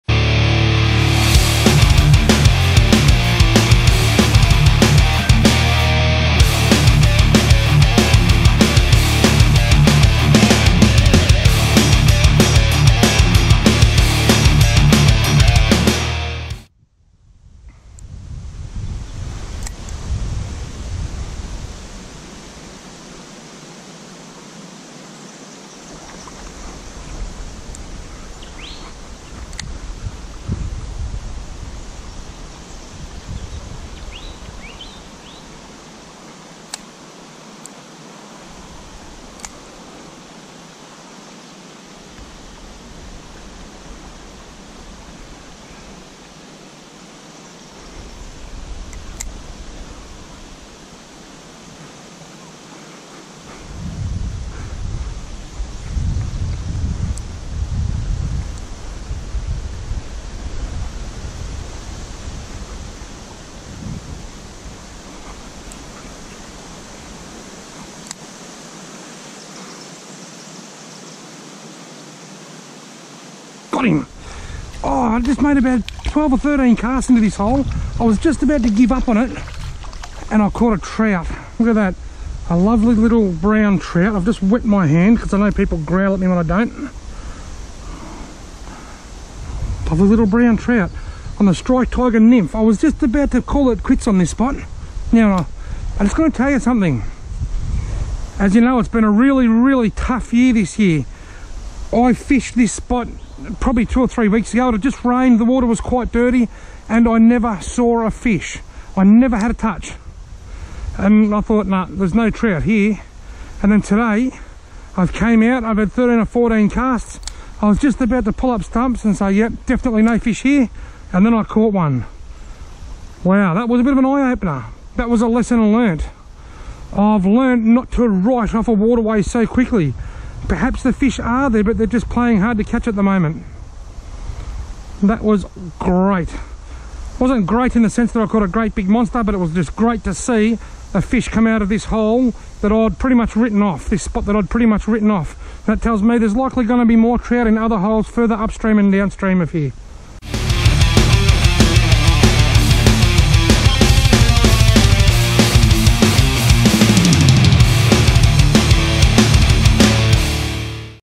Watch this before writing off a waterway
Before you write off a waterway as being "stuffed" make sure you watch this video first. I had fished it a couple of times already this season and done no good. Today, after a dozen or more casts I was about to write it off when a small brown trout came from out of nowhere to hit my Strike Tiger nymph soft plastic.
 Sometimes we all get a bit complacent, or perhaps a bit too over confident and I very well may have been doing that today myself when I had called this spot as being written off before I hooked up.
When trout fishing usually it is obvious when you fish your more reliable spots and see no fish that it is stuffed or not fishing well.
 The reason I have shared this short video is so that you can learn from this experience just like I did. This was definitely an eye opener for me when trout fishing. In fact, trout fishing is full of surprises and we never truly know everything. Little pearls of wisdom which only come from experience such as this video can be the best learning tools we have for trout fishing.